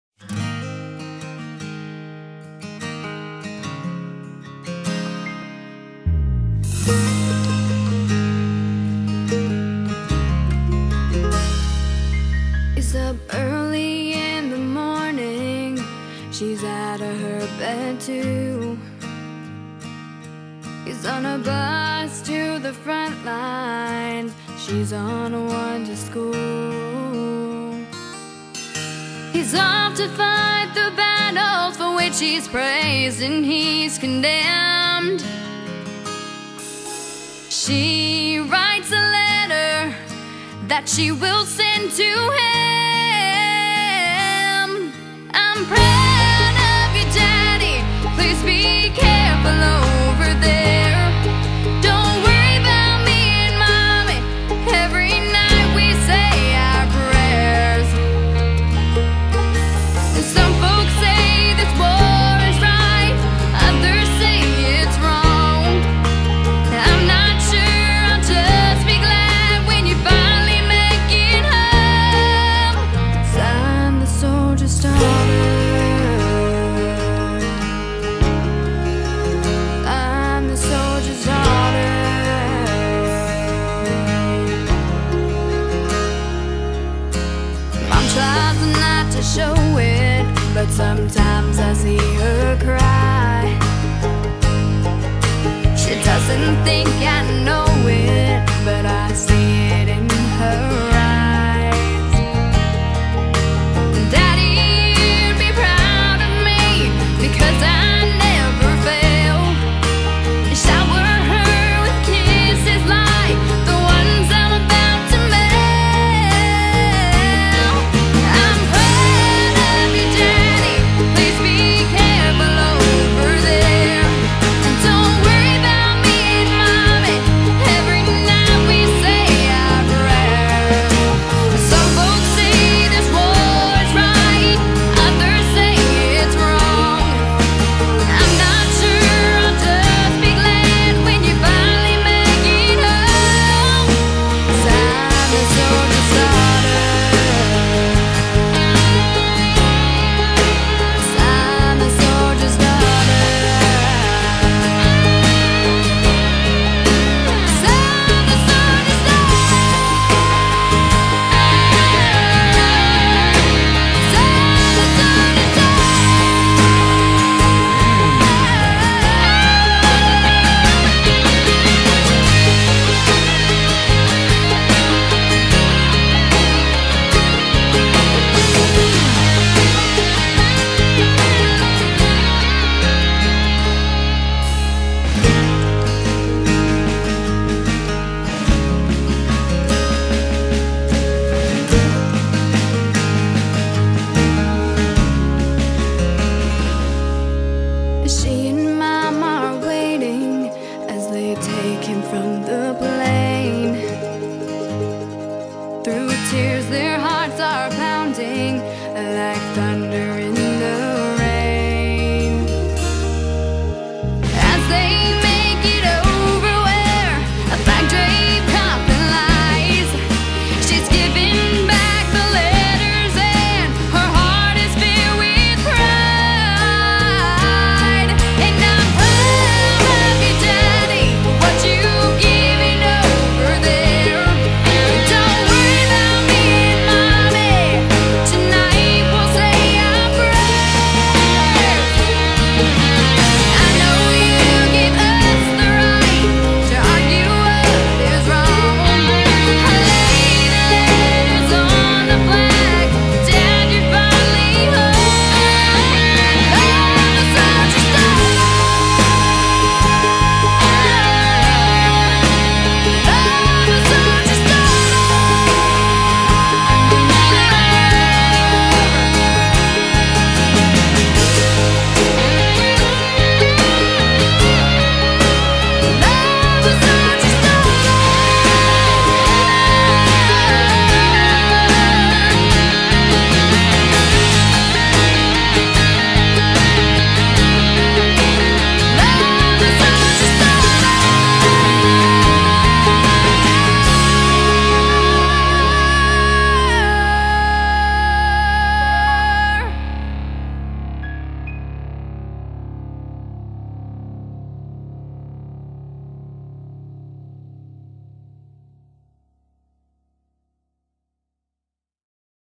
SSG Robert Thomas
North Texas PGR Escort on 09/25/2013